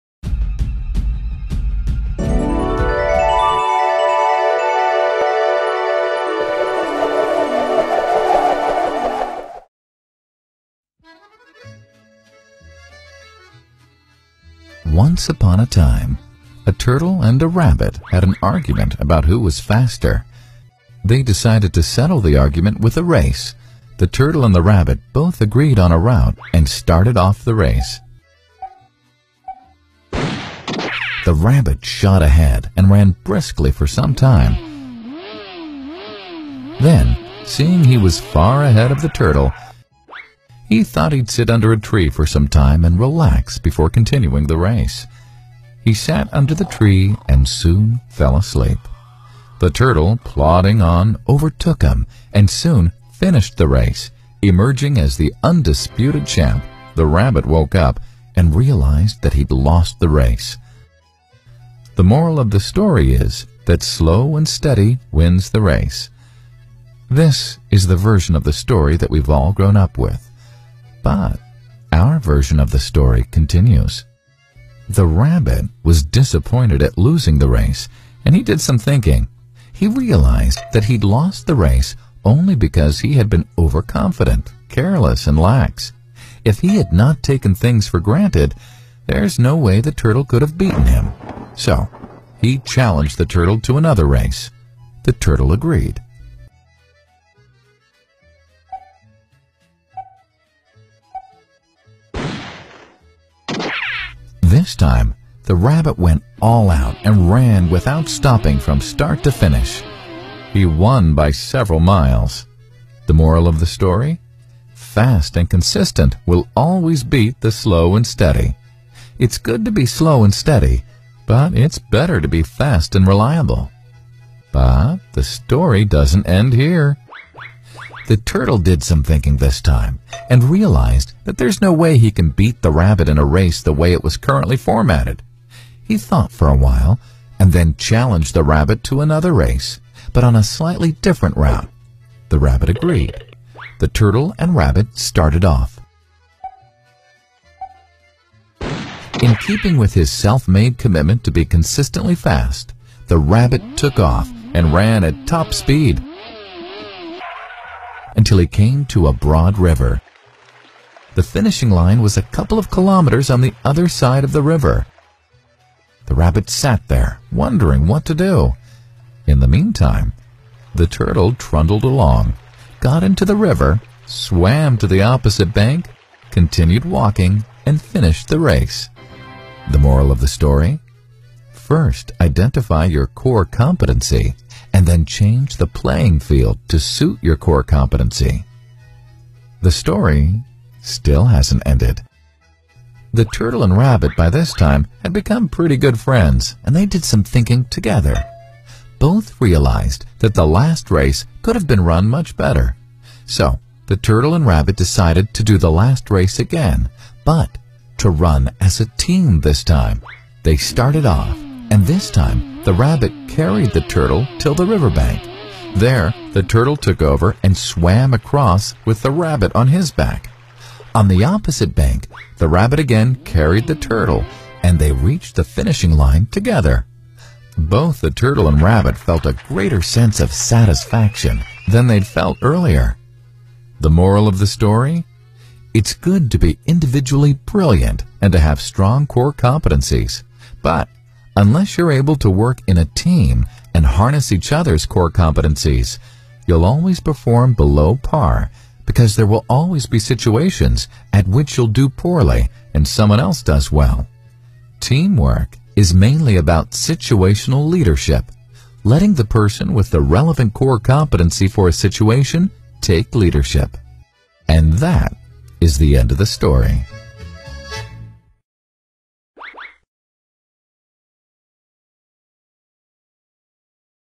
Rabbit and the Turtle
This story of the rabbit and the turtle has a different message altogether!! Curious?